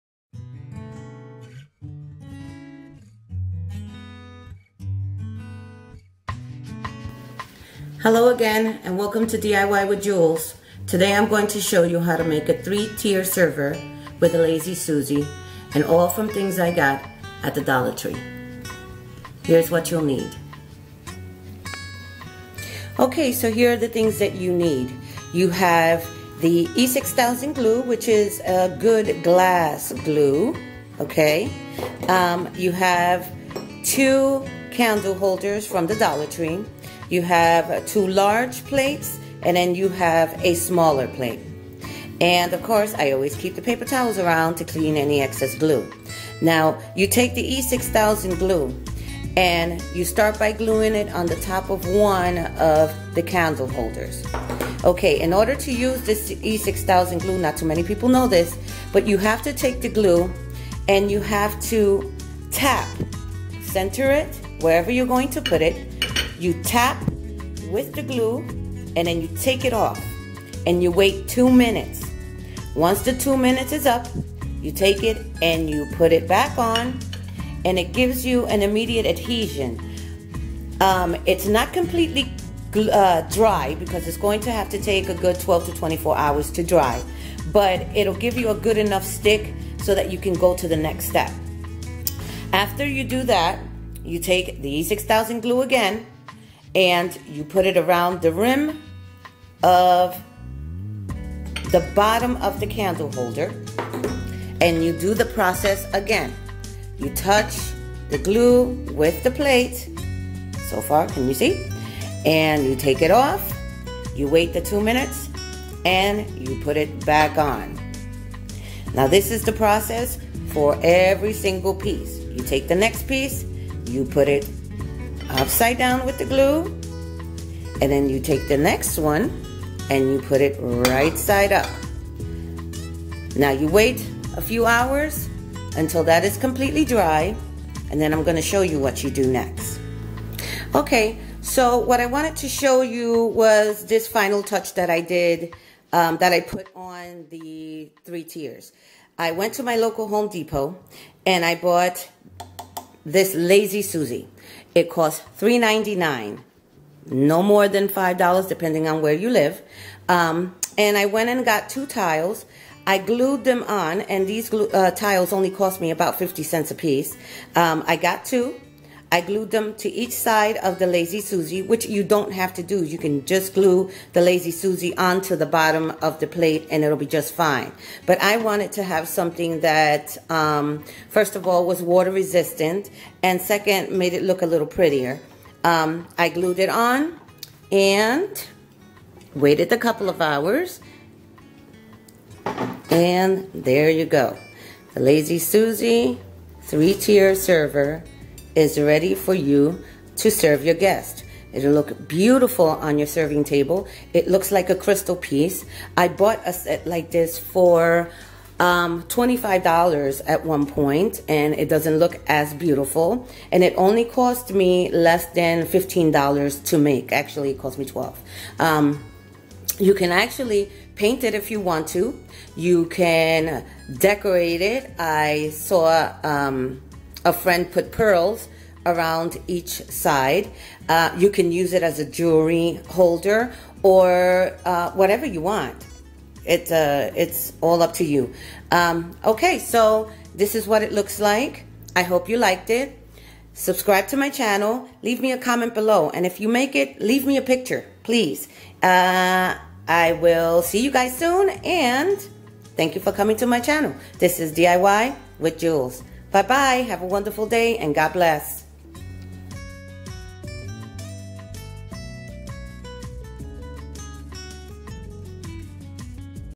DIY Three Tier Server
DIY
Wedding cake stand Or cupcake stand
Glass stand or jewelry holder
This video will show you how to make this inexpensive but elegant serving tray made in three tiers and with a lazy Suzie it will make a beautiful addition to your serving dishes